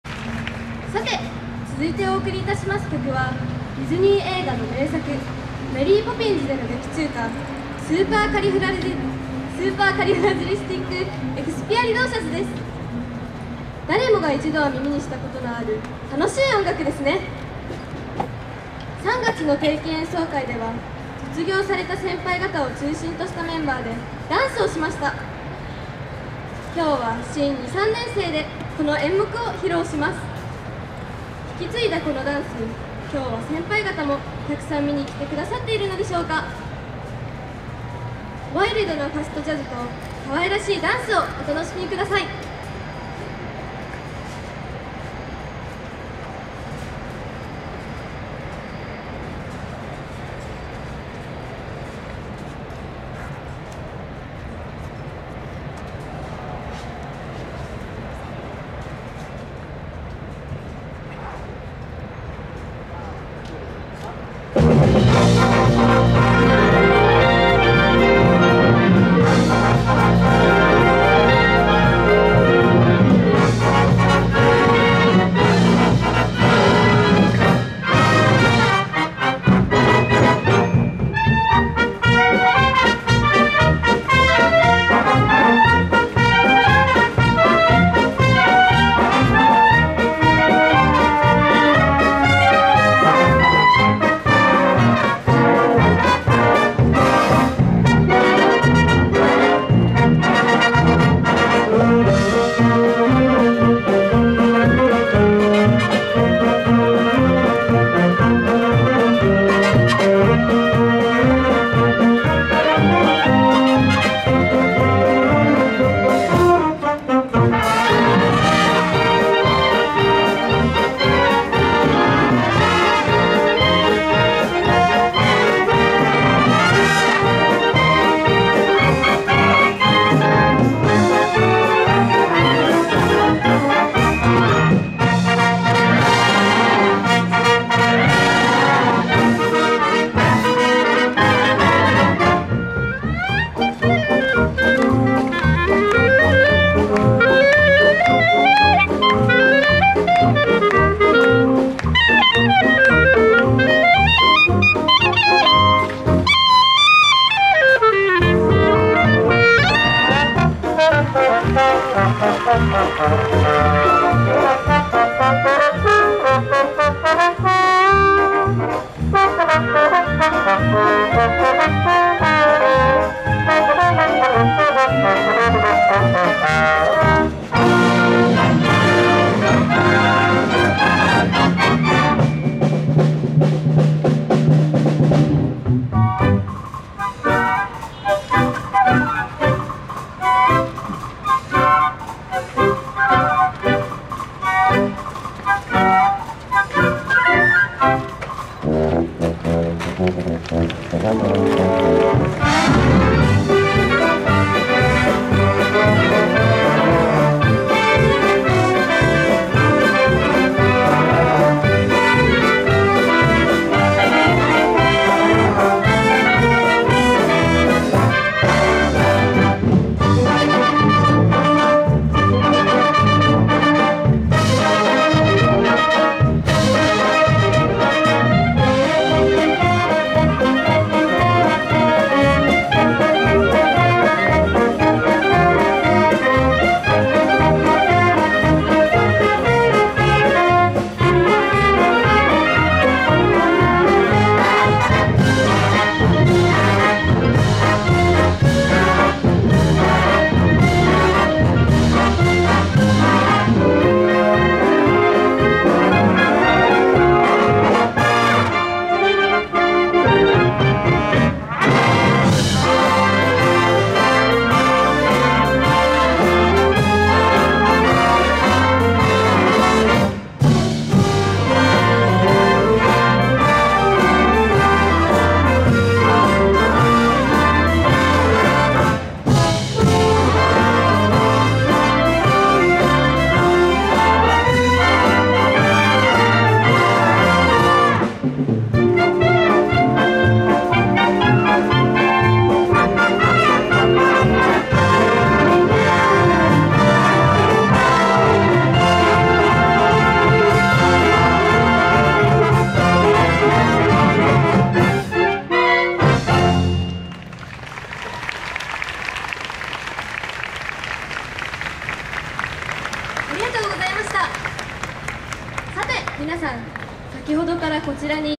八王子学園八王子高等学校吹奏楽部 ②／コンサートの森／第十三回 銀座柳まつり 2019
2019/5/05（令和元年5月5日）
第十三回 銀座柳まつり コンサートの森
【主催】全銀座会、西銀座通会、第十三回銀座柳まつり実行委員会
【共催】銀座通連合会、東京都吹奏楽連盟 　協賛企業多数
【後援】中央区